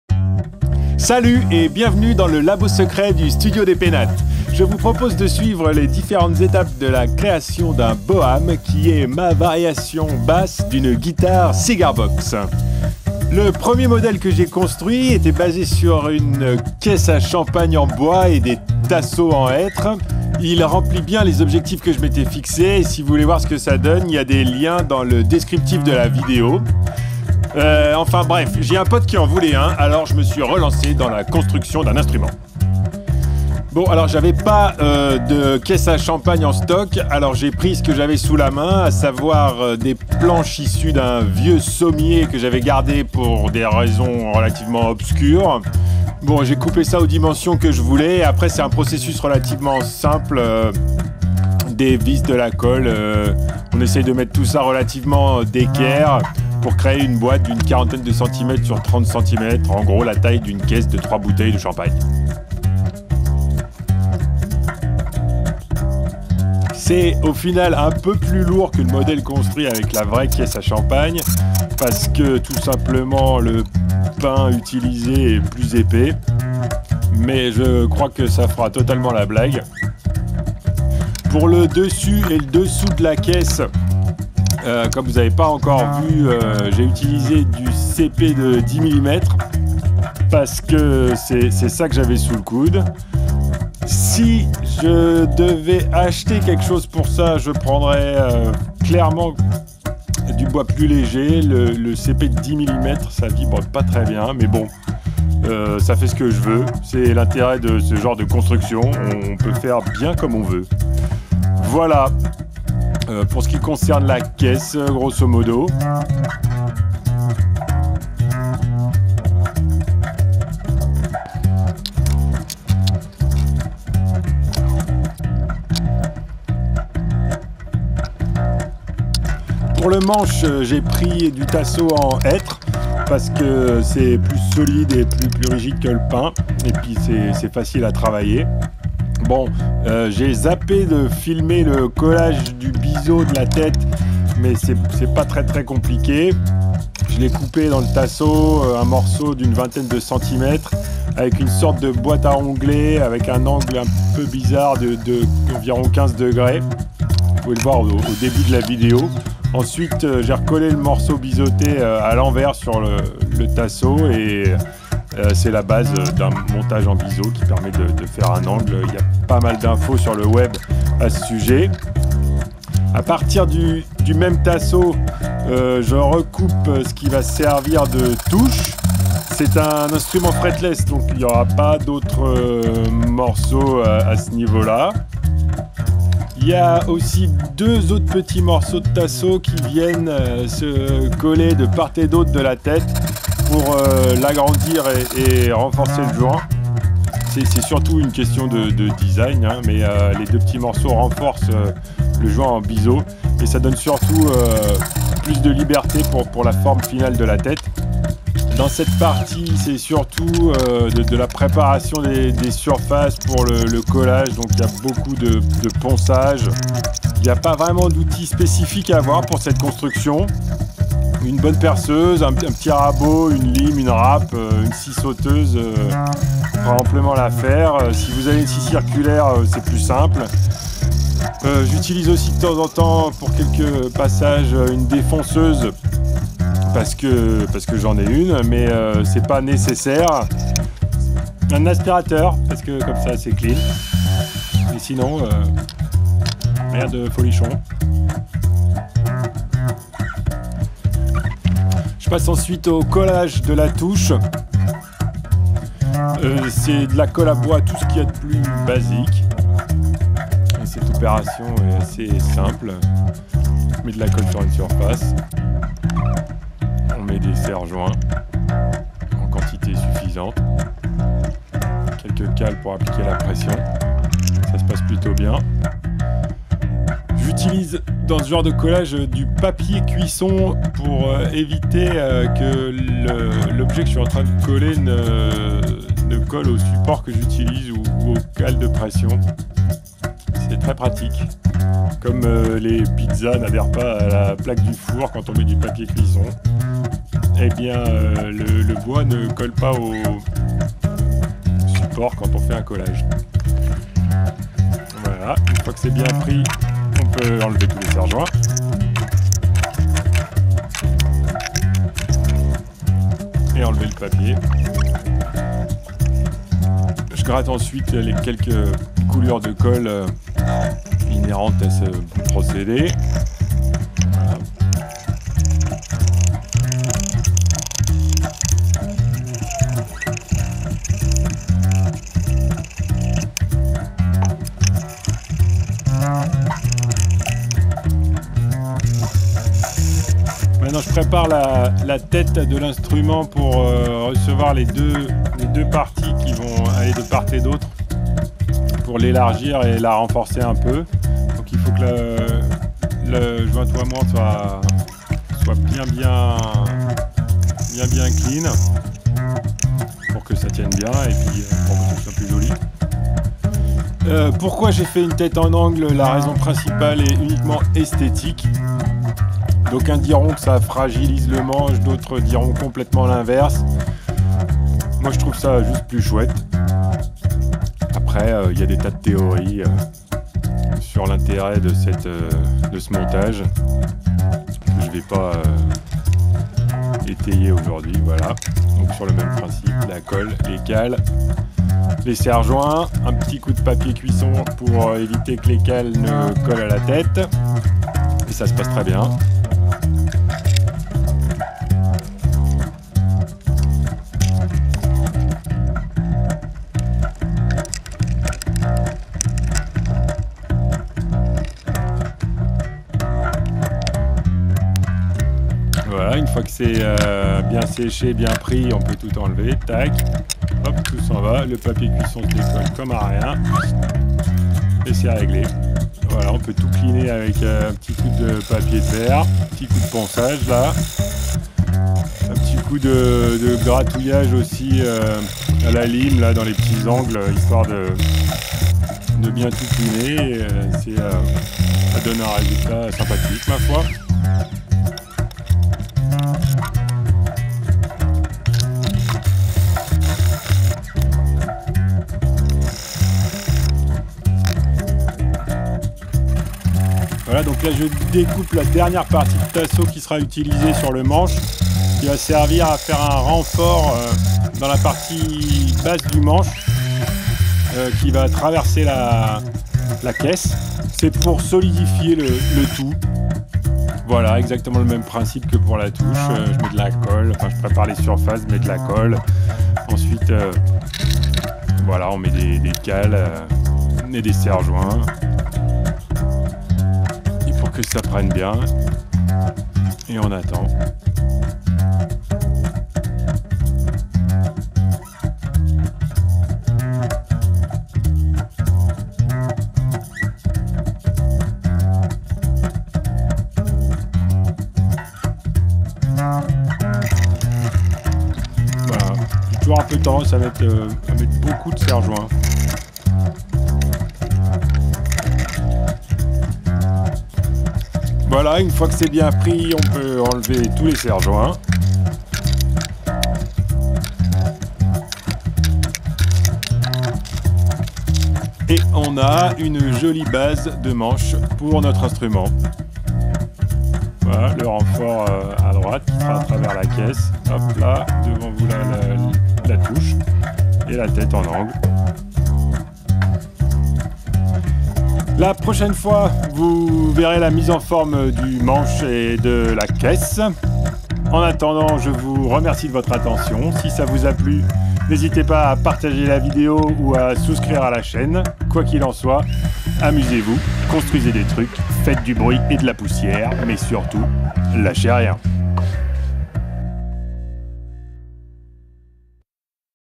Construction du J-Boam étape 1 sur 5
Dans le labo secret du Studio des Pénates, construction d'une basse inspirée des guitares "cigar-box".
Le premier modèle du genre est visible en version acoustique,
et là avec un micro magnétique home-made.

Fabriqué et enregistré au Studio des Pénates.